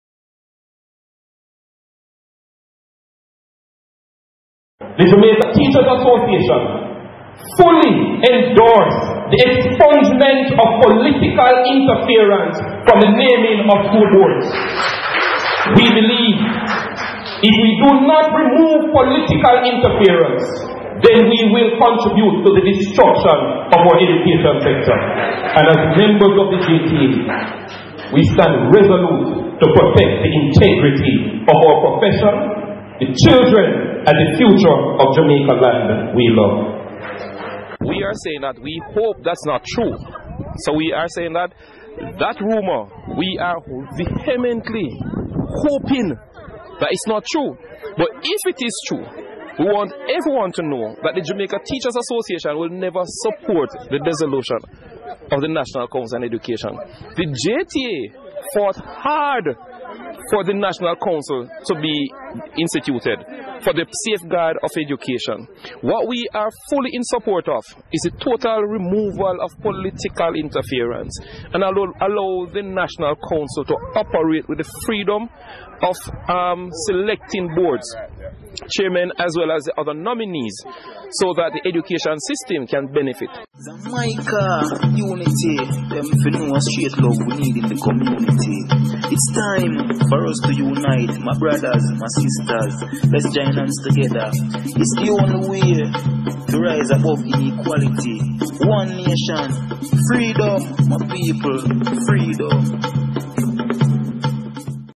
Jamaican Politicians Playing Games With Our Children's Education?
"Fury over school board appointments at STETHS. President of the Jamaica Teachers Association, JTA, Winston Smith says he will not sit by and allow politics to determine how persons are appointed to school boards."

Truth  Speaks Freely on XU Truth TV